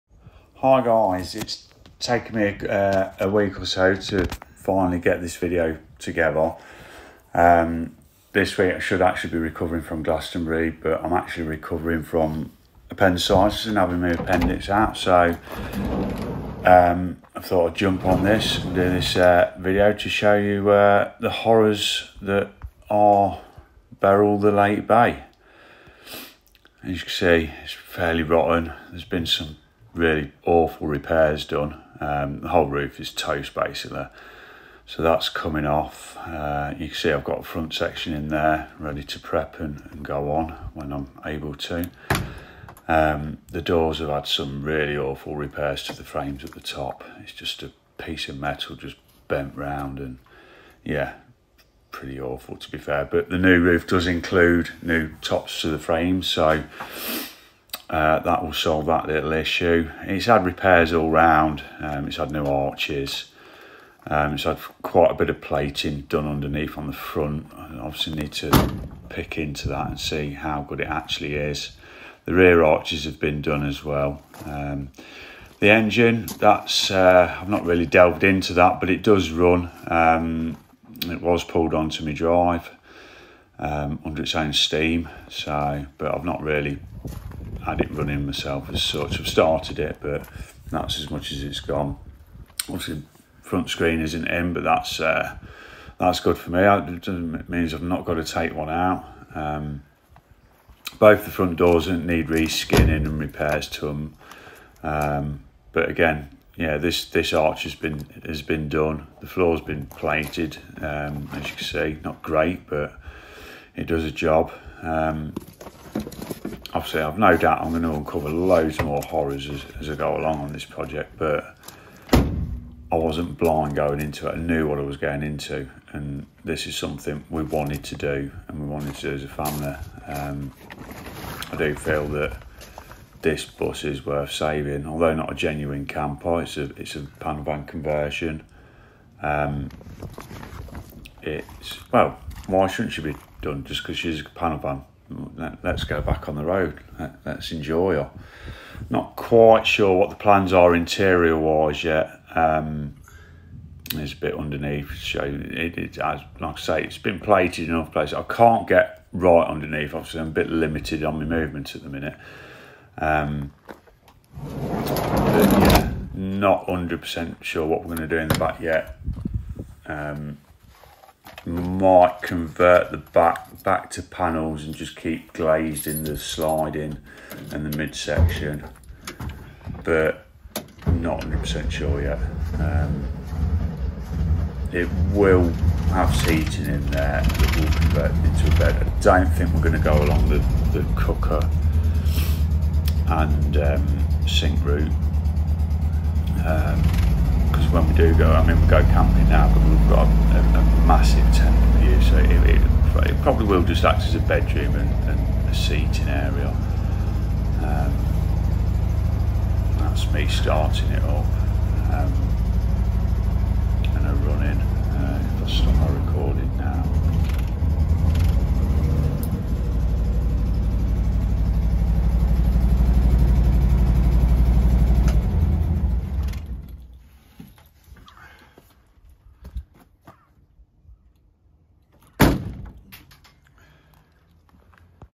VW Bay window restoration. Introduction to Beryl our family project. Type 2 classic VW
Quick introduction to Beryl our 1979 VW bay window bus.